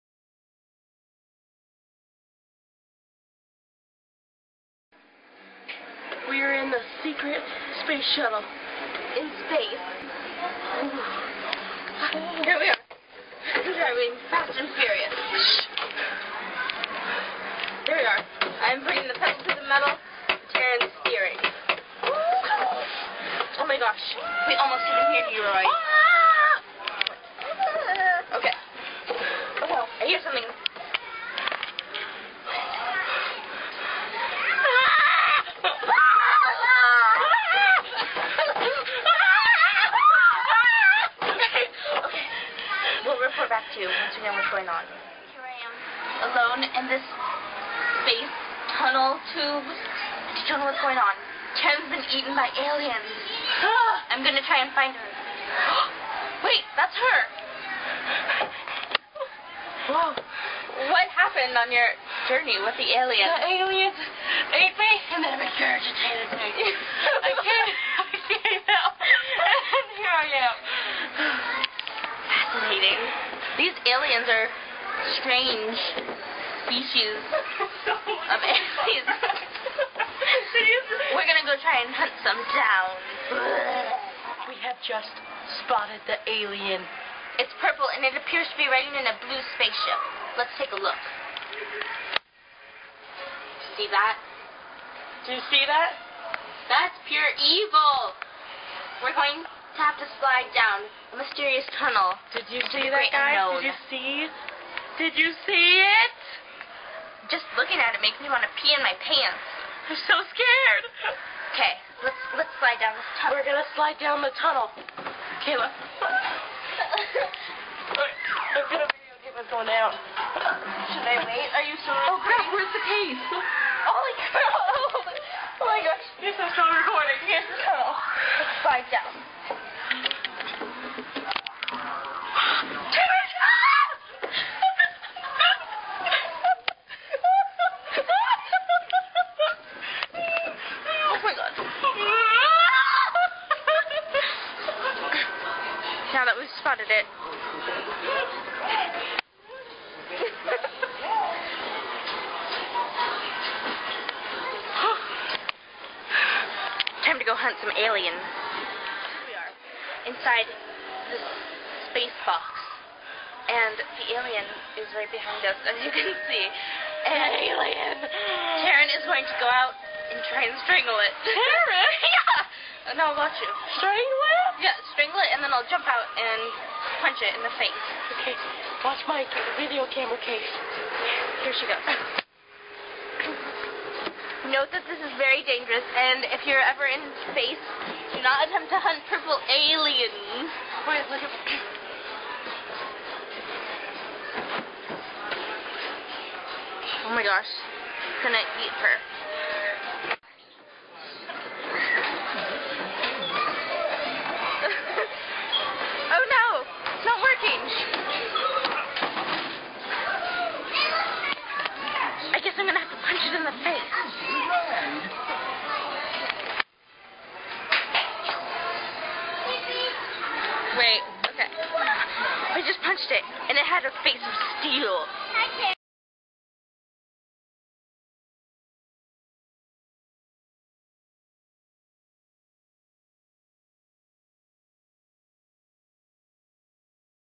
The Purple Alien
This is Kayla and I on a ferocious alien hunt!